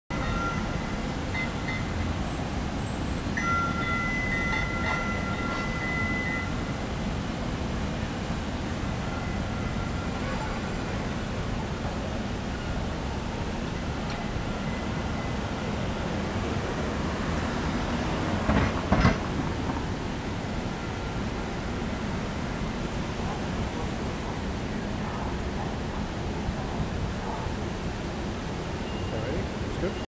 An Atlanta Streetcar Arriving At The Woodruff Park Stop, USA
Atlanta streetcar arriving at the Woodruff Park stop, Atlanta, Georgia, USA.